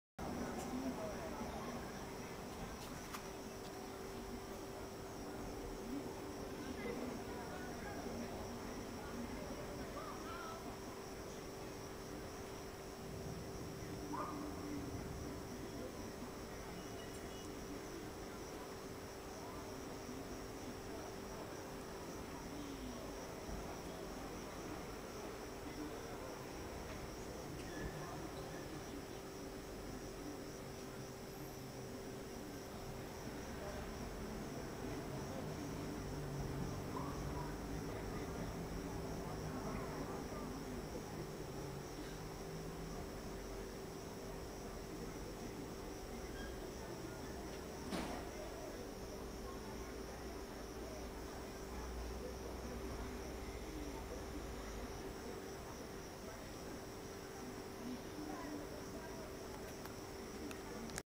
La sera prima di Ferragosto (seconda parte)
Meno male che il maltempo si era fermato lì dalle parti di Itri, a Formia non è arrivato, da me  una serata serena. Il cielo ha minacciato per un'ora. Oggi pare che il tempo sia favorevole.